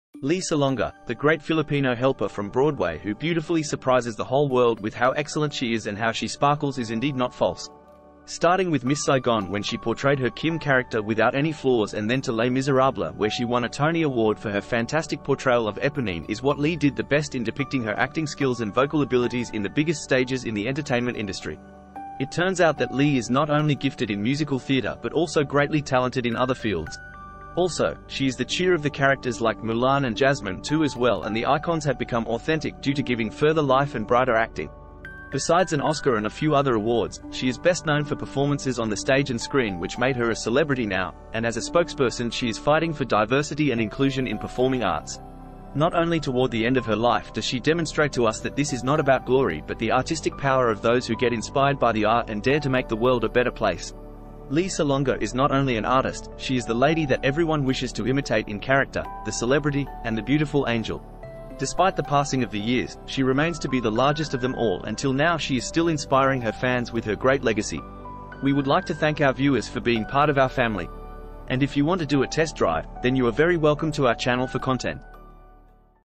"The Phenomenon of Lea Salonga"
Experience the unparalleled talent and remarkable journey of Lea Salonga, a true icon in the world of Broadway and Filipino entertainment. From her groundbreaking role in Miss Saigon to her Tony Award-winning performances, Lea Salonga has captivated audiences with her powerful voice and magnetic stage presence. Discover the phenomenon of Lea Salonga and get an inside look at her incredible career that has spanned decades.

Don't miss out on this exclusive profile of one of the most respected and beloved performers in the industry.

Watch now and witness the magic of Lea Salonga as she continues to inspire and enchant fans around the globe.